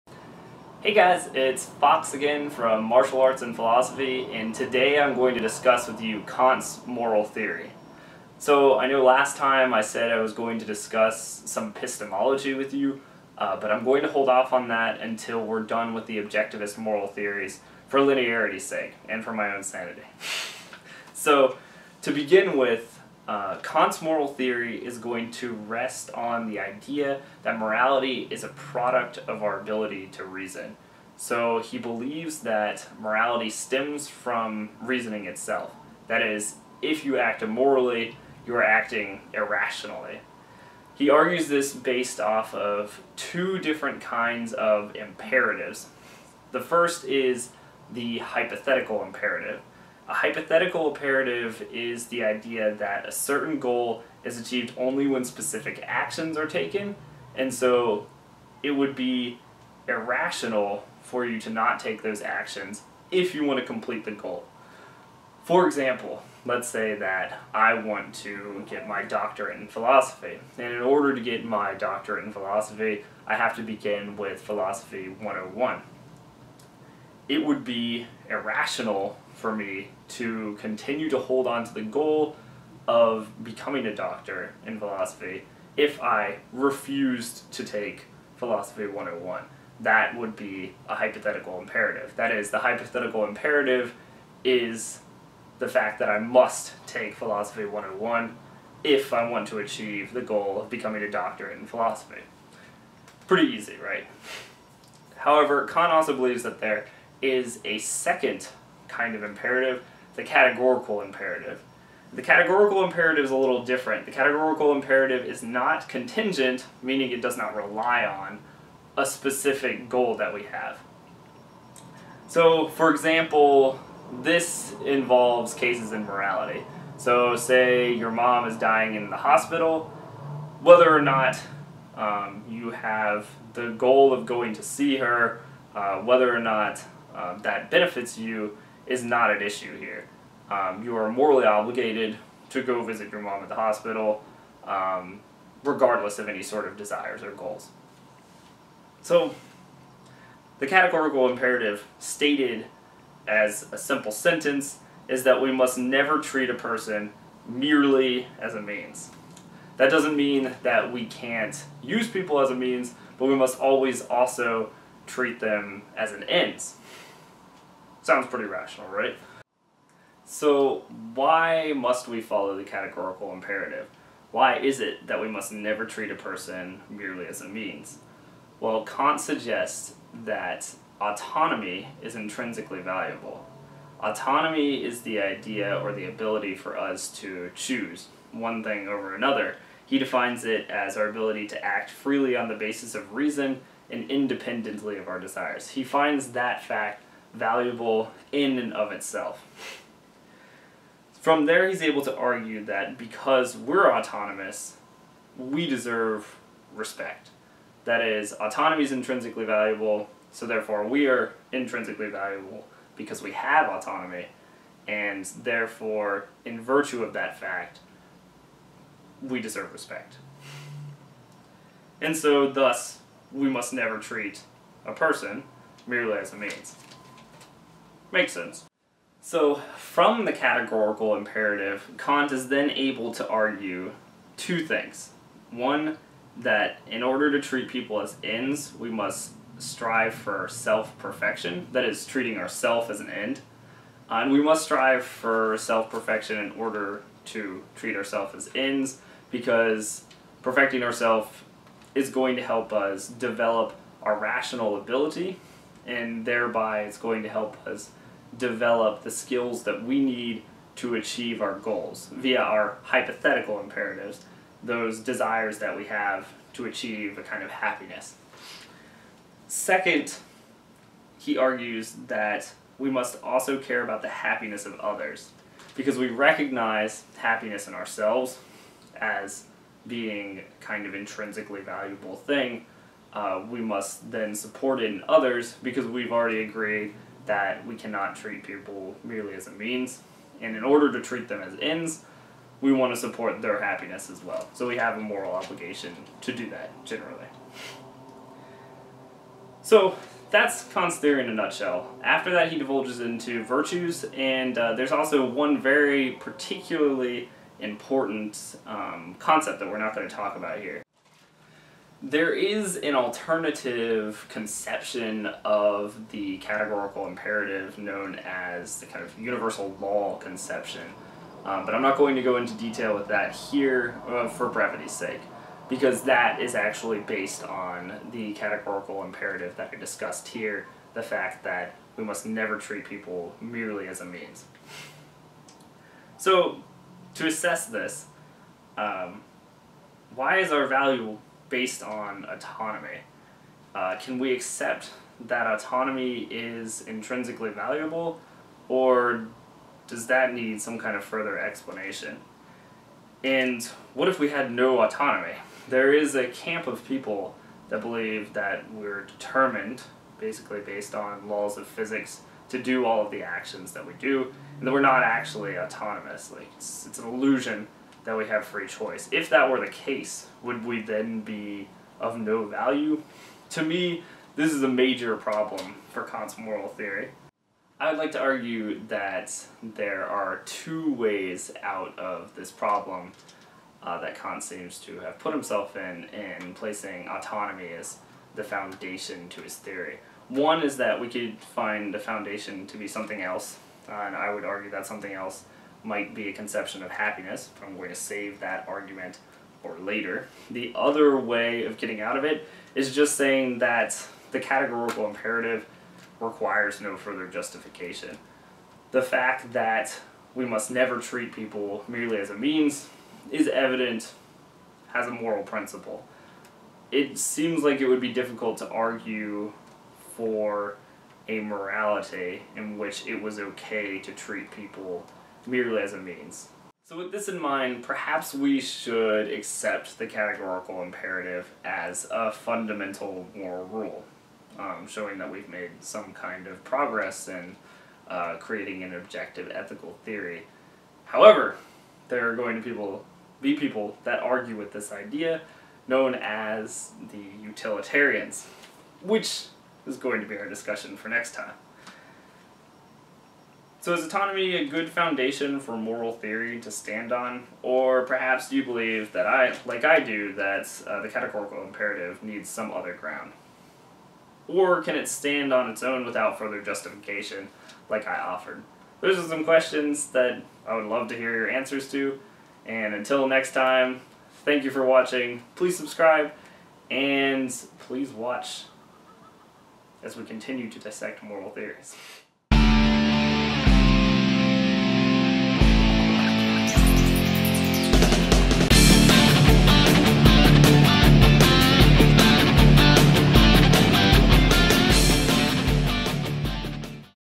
Can We Base Morality on Respect?
This video is a short critical overview of Kant's primary principle
regarding morality: the Categorical Imperative. Can the concept
of respect justify a universal morality?